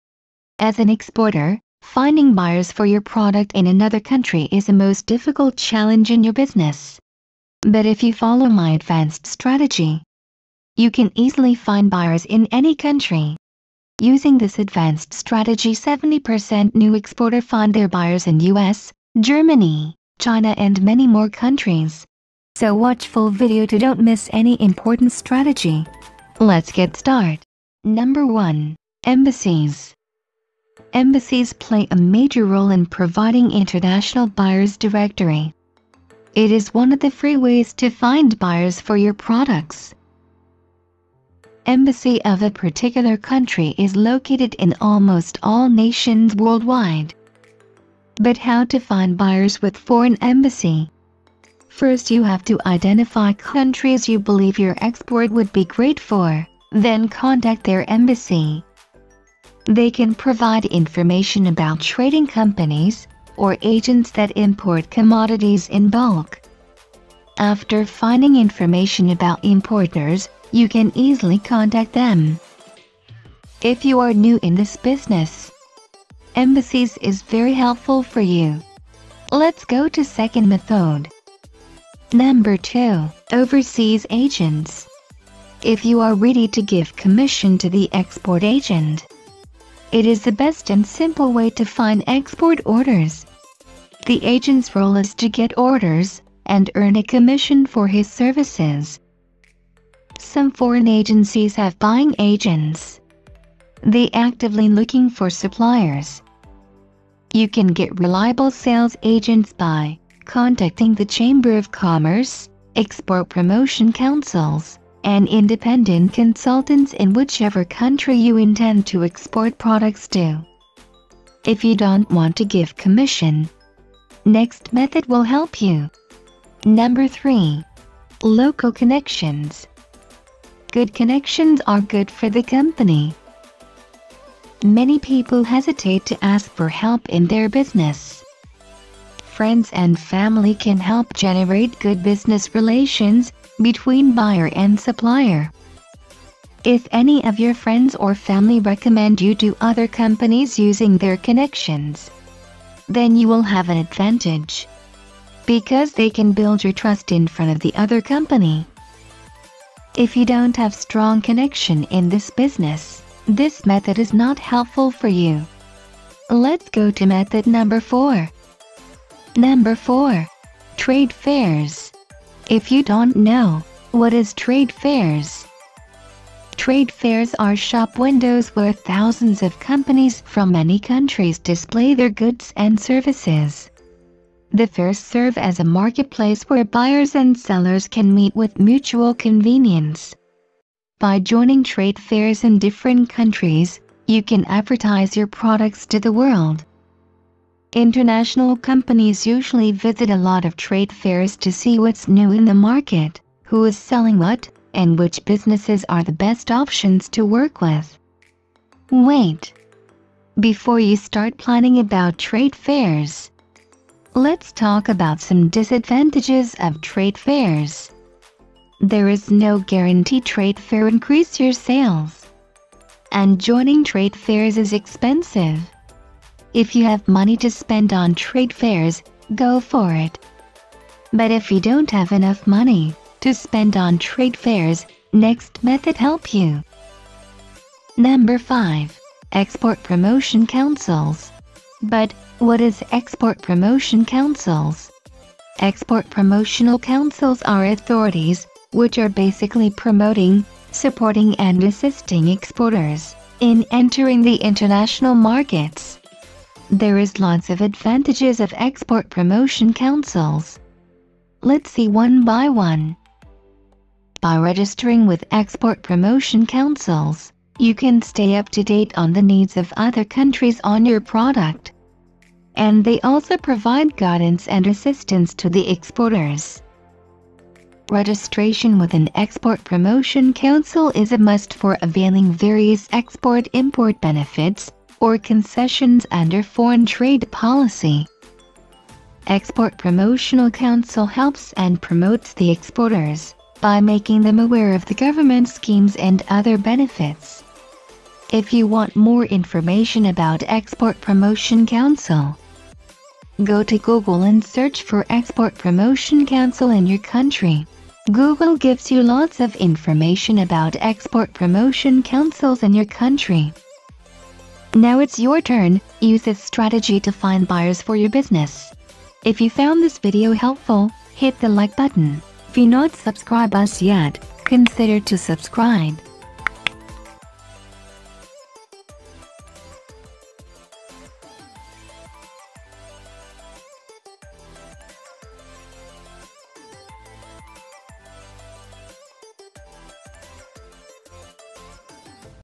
Find Buyers For Export In 2022 (Fast!)
Find Buyers For Export In (2021) Is Not Difficult If You Know this (Advanced  Strategies)

These Are The Same Strategies That I Used To Find Buyers For My Export Business In (2018)

With that, here's what you'll learn in today's video:

(No.1) Embassy

First, I'll show you  How To Find Buyers In Abroad With Embassy
Using Embassy You Can Find Buyers List And Company Data

(No.2) Overseas Agent

Our second Method Is Overseas Agent
If You Are Ready To Give Commission To  The Agent  This Method Is Very Easy And Helpful. But If You  Don't Want Give Commission To The Agent Next Method Will Help You

(No.3) Local Connection

Good connections are good for the company. If You Have Strong Connection In This Business. Then it's Not Difficult To Find Buyers For Your Product

(No.4) Trade Fairs

By joining trade fairs in Different country's, you can advertise your products to the world. But  joining trade fairs Is expensive. If You Have Money To Spend On Trade Fairs, Go For It.

(No.5) Export Promotion Council

Export promotional Councils  are authorities, which are promoting Exporters, In entering the International markets.
There Is Lots Of Advantages Of Export Promotion Councils. Lets See One By One In Video.

After Watching This Video You Know How To Find Buyers For Exports.

Its Your Time To Use This Strategies  And Grow Your Import Export Business.

So Go And Find Your Buyers.  (Best Of Luck)

Every single Video  on my channel is made with Love and Hard